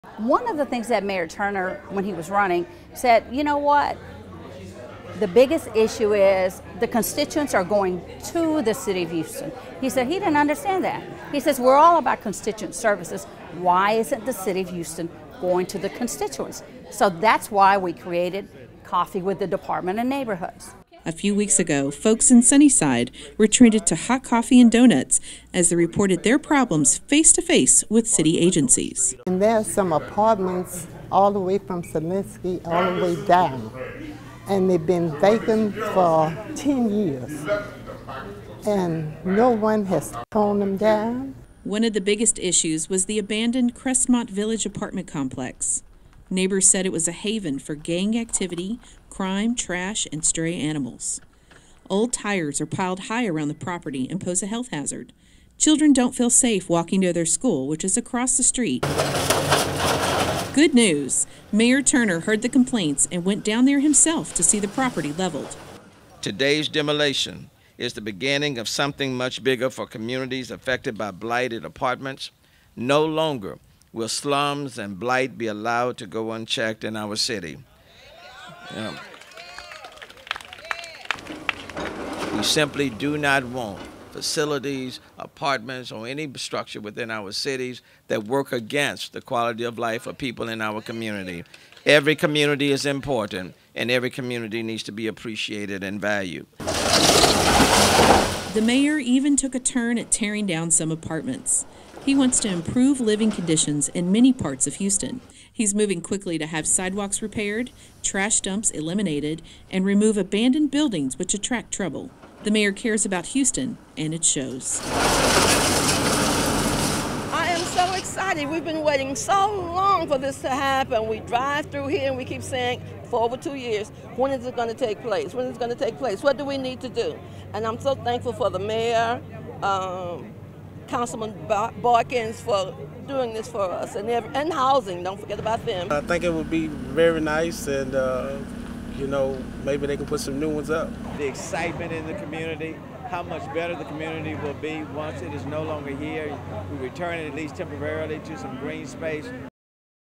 Demolished: Crestmont Village Apartments No Longer a Hazard
At a recent "Coffee with Neighborhoods" meetup, residents from the Sunnyside community spoke about the Crestmont Village Apartments, which have been vacant for nearly a decade.

Mayor Sylvester Turner heard their complaints and was present at the demolition of those dilapidated apartments.